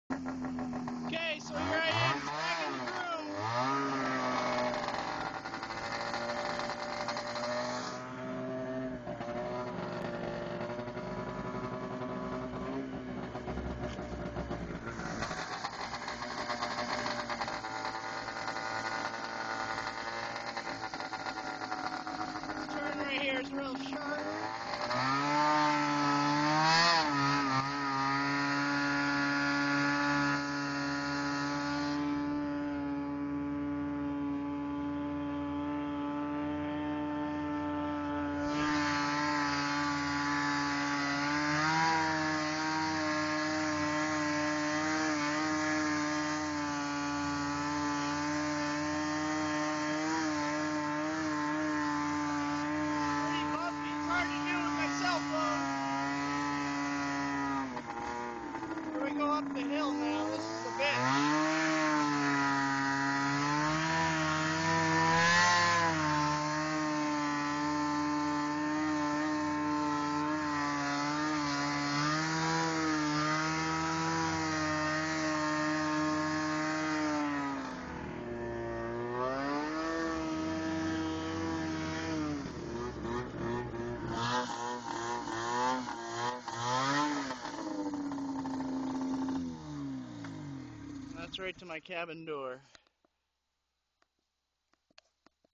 grooming my snowmobile trail at camp.3GP
grooming with the ditchbanger at camp